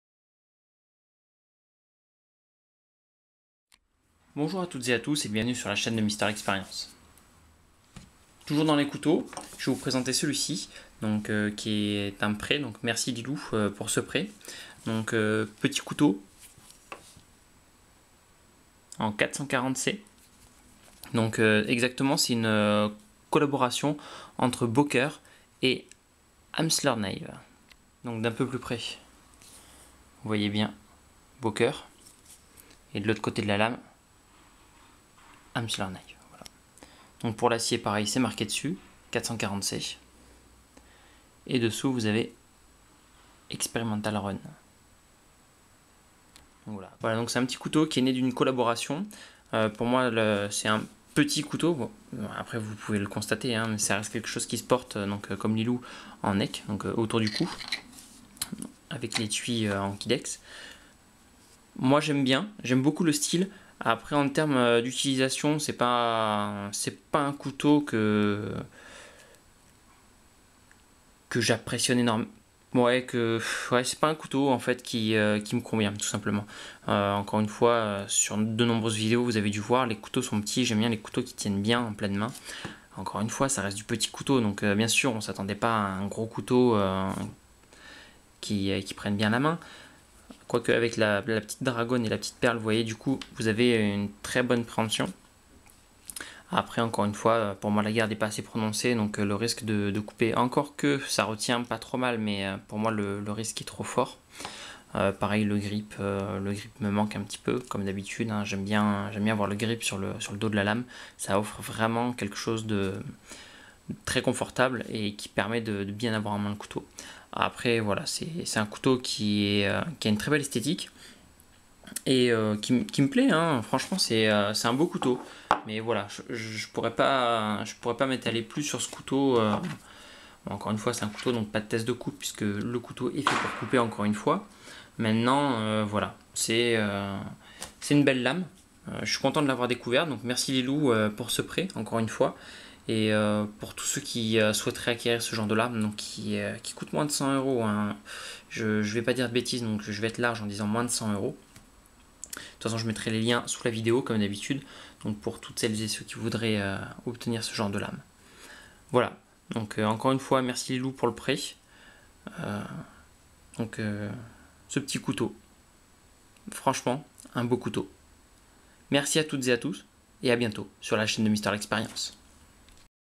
Böker Amsler Knives - © Mrlxp ® (Fr)
Petit couteau né d'une collaboration entre Amsler Knives et Böker.
Très bon couteau et pratique.

Couteau de Cou Boker Plus Mini Slik Pikal Acier 440C StoneWash Manche Titane 02BO230 - Livraison Gratuite

Couteau de cou Boker Plus Mini Slik Pikal à lame fixe - Longueur totale 12.38cm - Longueur de lame 5.39cm - Construction d'une seule pièce type plate semelle - Lame en acier 440C stonewash - Plaquettes du manche en titane

© Mister l'expérience ®